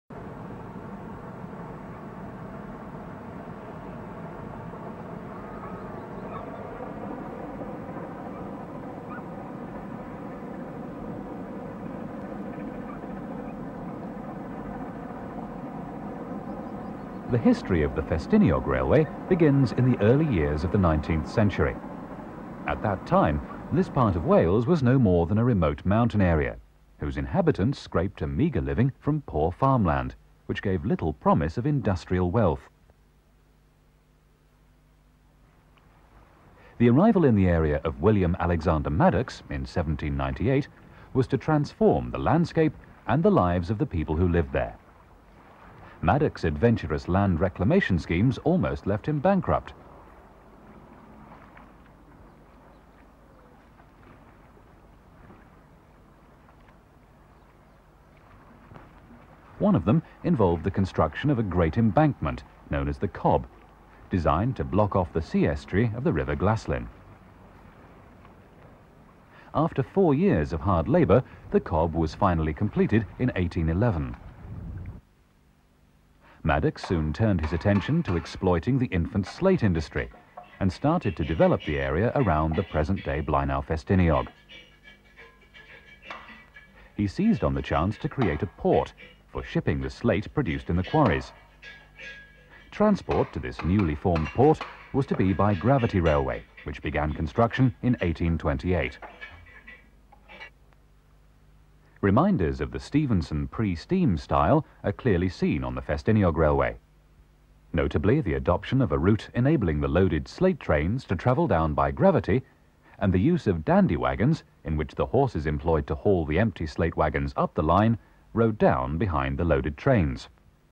FFESTINIOG RAILWAY - 125 YEARS OF STEAM - THE MOST SPECTACULAR RAILWAYS OF EUROPE
The Ffestiniog Railway in North Wales was originally constructed to convey slate from the quarries in Blaenau Ffestiniog to Porthmadog. The line was built on a steep incline to facilitate the use of gravity trains. In 1863, the increase in traffic forced the owner to consider different forms of motive power. After many trials and much development work, a special design of locomotive incorporating power bogies took to the narrow gauge rails. During the Spring of 1988, the railway celebrated 125 years of steam power with a gala weekend spanning 3 glorious days which saw the return to service of the newly restored Fairlie double engine Merddin Emrys, vintage trains hauled by Prince, visiting W.H.R. Locomotive Russell, and much more besides. Our cameras were there to record the event in full, and this film, produced in conjunction with, and the full cooperation of the Ffestiniog Railway Society includes exclusive footage shot on the double headed run up the line with Blanche and Mountaineer.